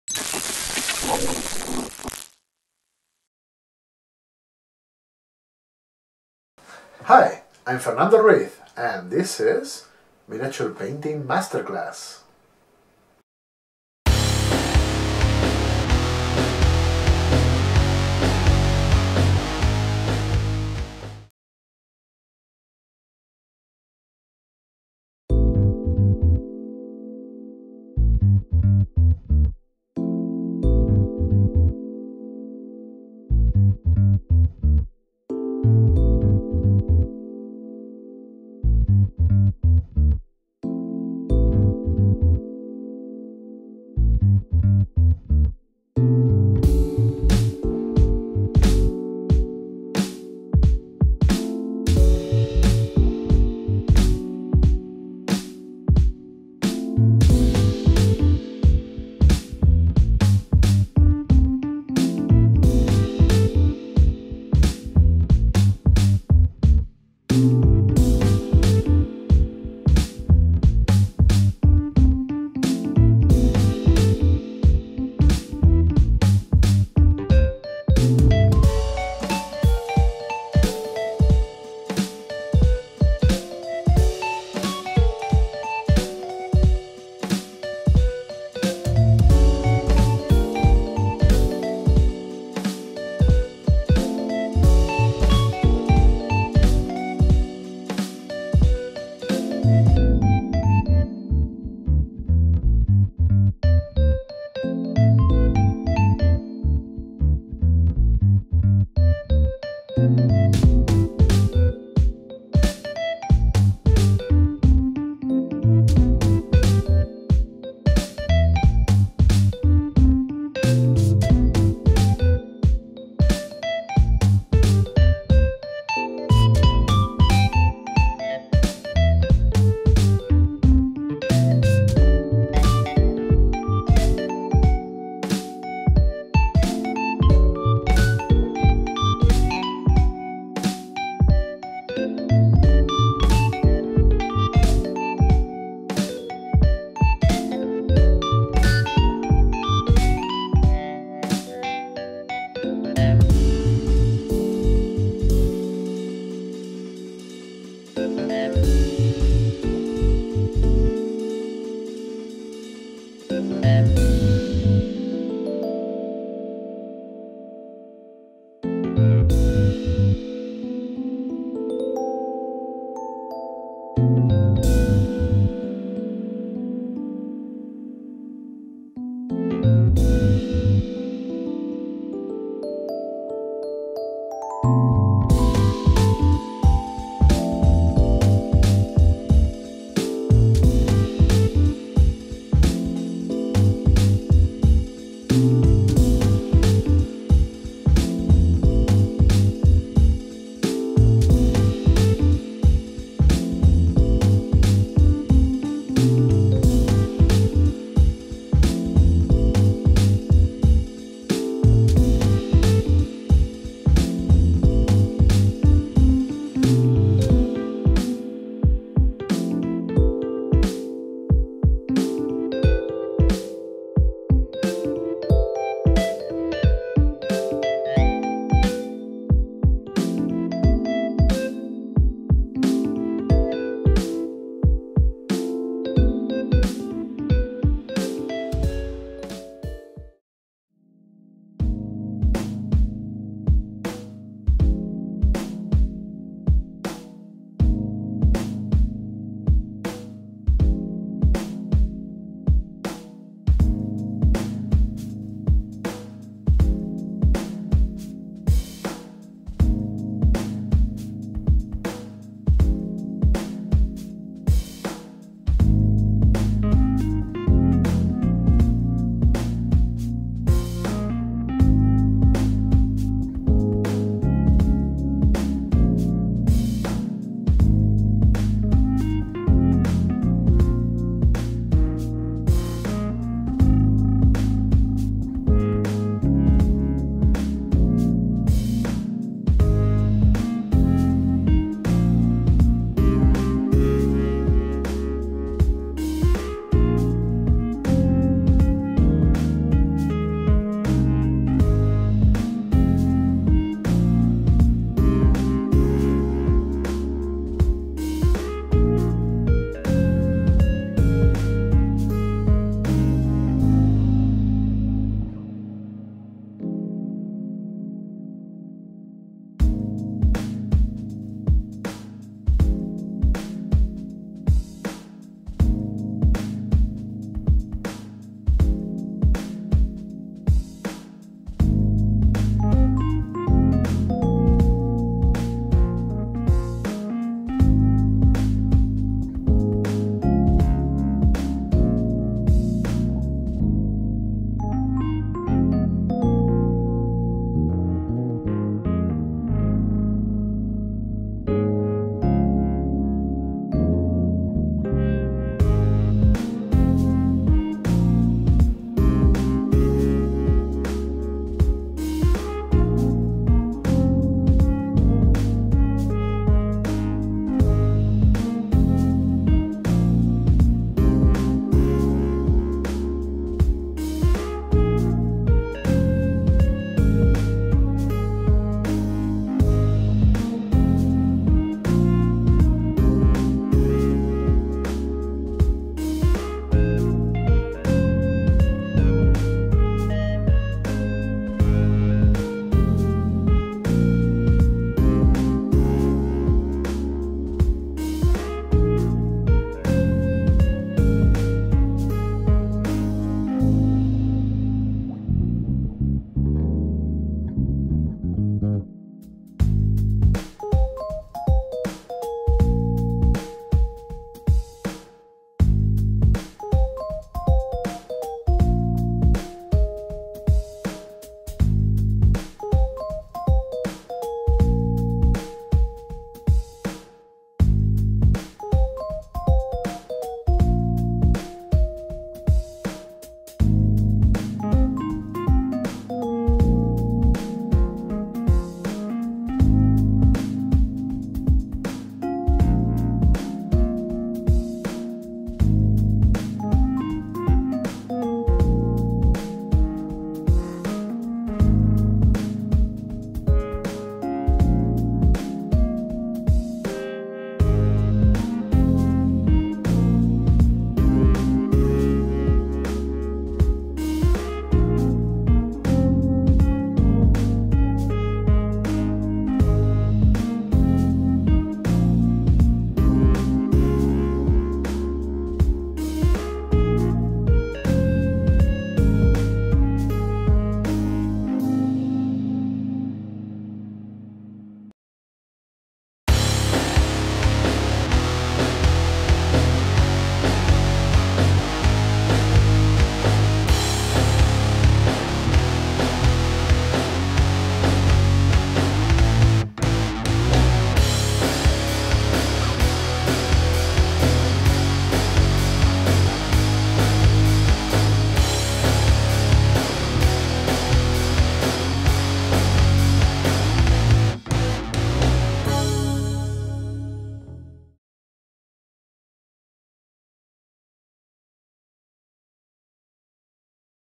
Painting a Roman helmet with Vallejo Metal Color and Game Color inks by Fernando Ruiz modificado
In this first tutorial from our collaboration with Miniature Painting Masterclass and Fer Miniatures, we explore the possibilities of combining Metal Color paints and Game Color Inks from Acrylicos Vallejo in the helmet of a Roman Aquilifer bust. More videos to come!

Artist: Fernando Ruiz

🎨 Colors used:

⚙ Metal Colors
Burnt Iron 70.721
Chrome 70.707
Gold 70.725

🖍 Game Ink:
Yellow 72.085
Blue 72.088
Red 72.086
Green 72.098
Violet 72.087
Sepia 72.091

Our appreciation to MPM and Fer Miniatures for the video production.